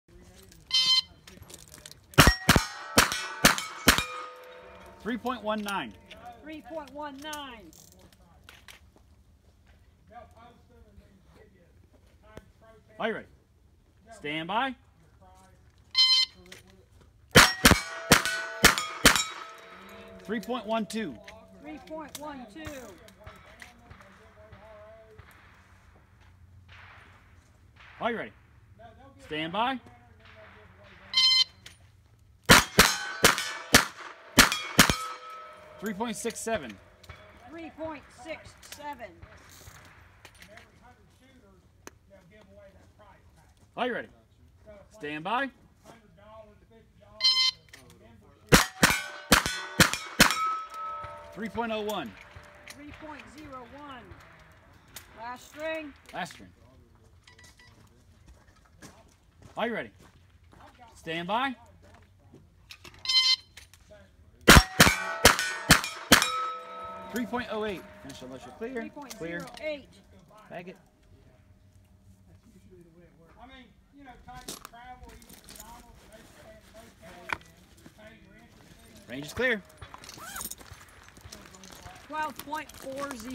Cory Ash, champion shooter
Cory Ash competes in the Steel Challenge Shooting Association’s 2017 Kentucky Steel Challenge State Championship in Wilmore, Kentucky, Nov. 10-11. (Video courtesy of Cory Ash)